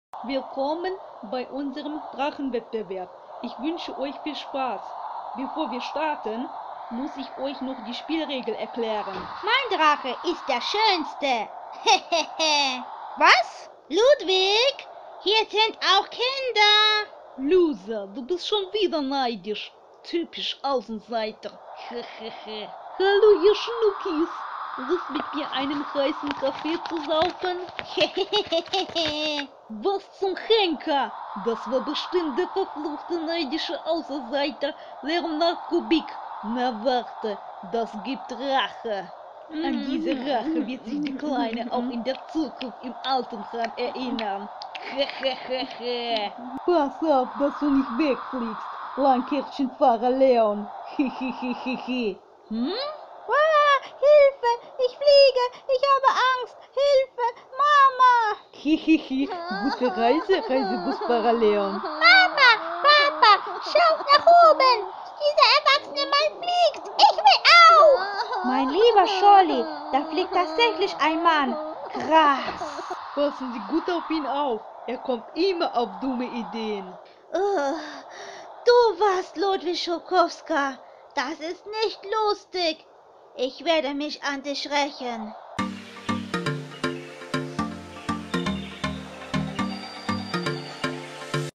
Comic: Die Abenteuer von Reisebusfahrer Leon Folge 33: Stürmischer Ausflug-Deutsch With English sub
Auch in der Corona-Pandemie erlebt der Reisebusfahrer Leon mit seinen Kollegen viele Abenteuer.
Idee, Sprecherin und Illustrationen: Sandra Schwarz